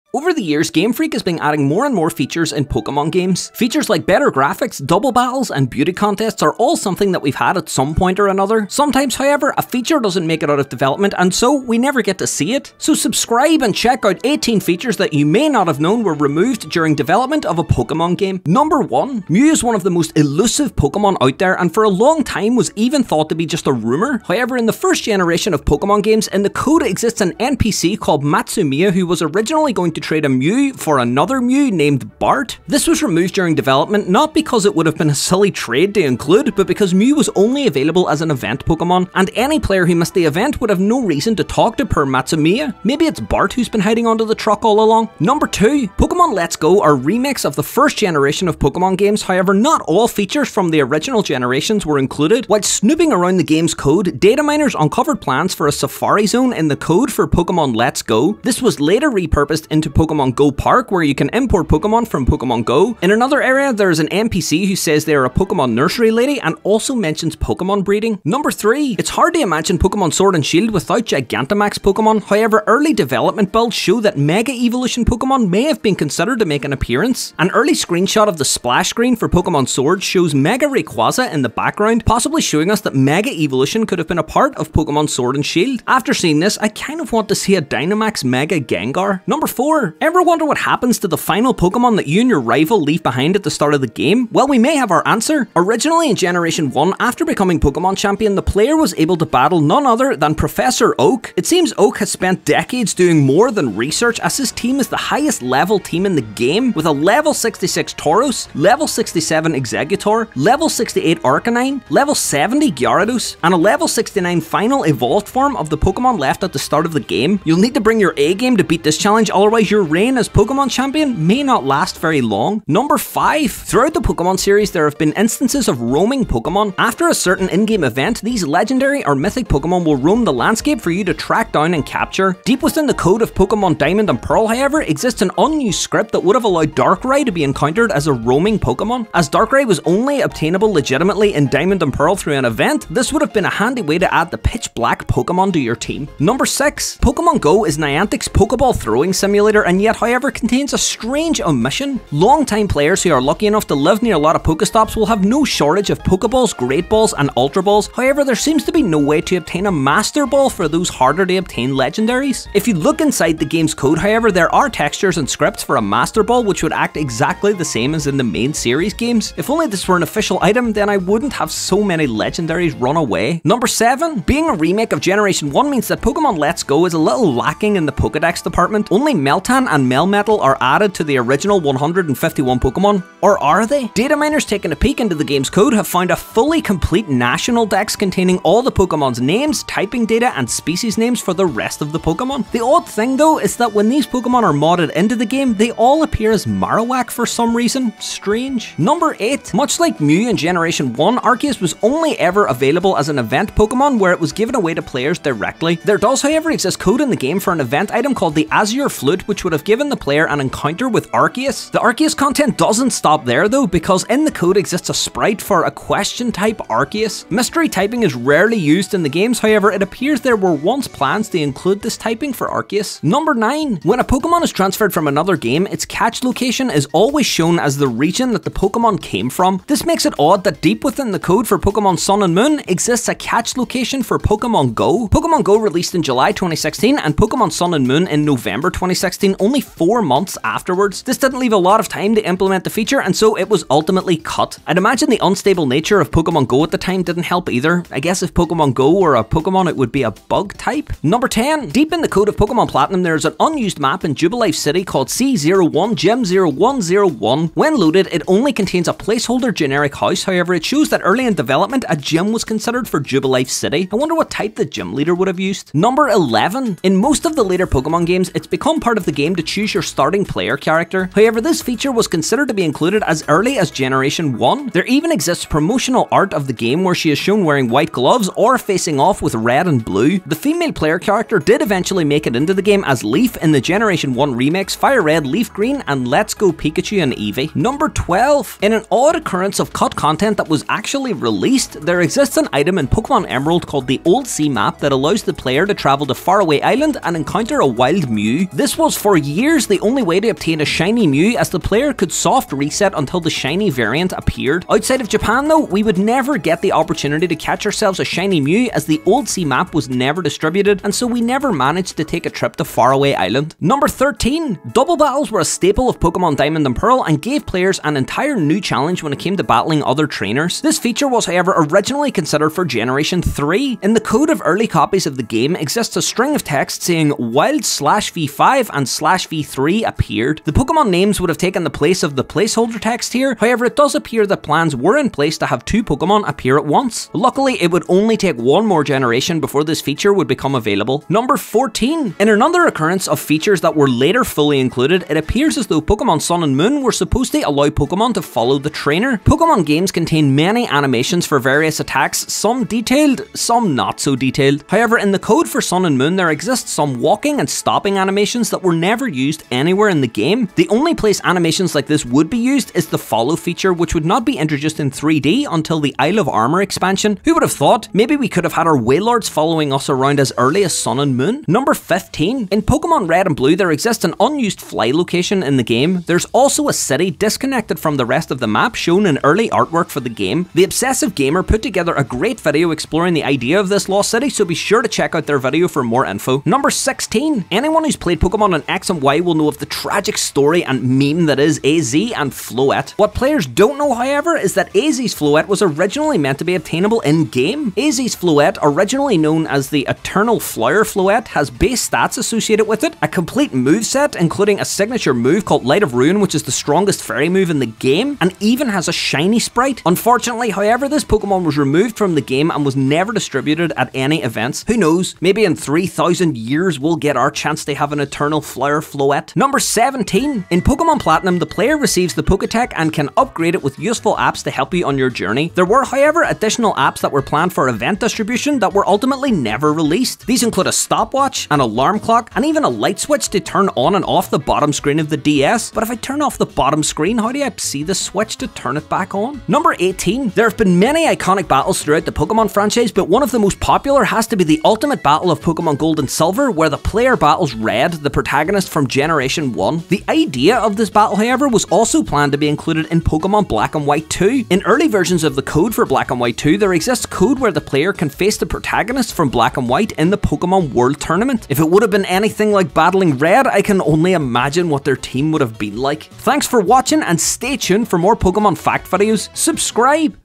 GameFreak REMOVED these 18 Pokemon Features!
Did you know any of these removed features? Over the years Pokemon have introduced lots of features into their main series of games, however sometimes beta features never get released leading to some Pokemon cut content.

In this video we list 18 removed pokemon features you might not know. Let me know if you want to see more cut content videos in future!